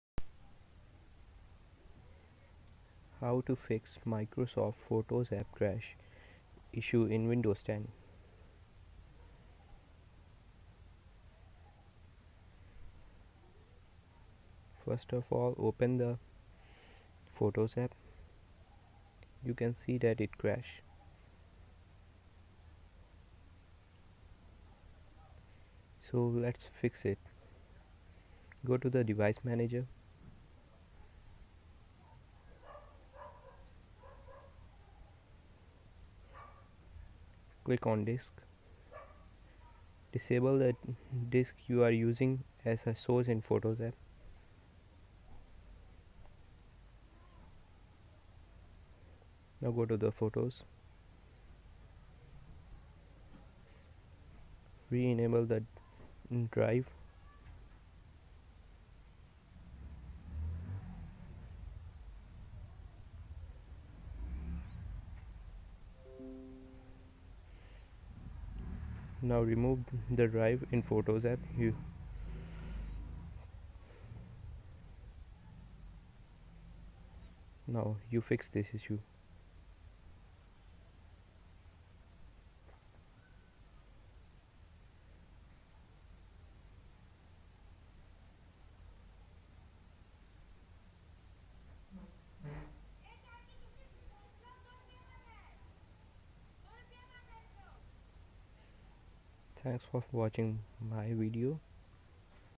Microsoft photos app crash fix
How to fix Microsoft photos crash issue in windows 10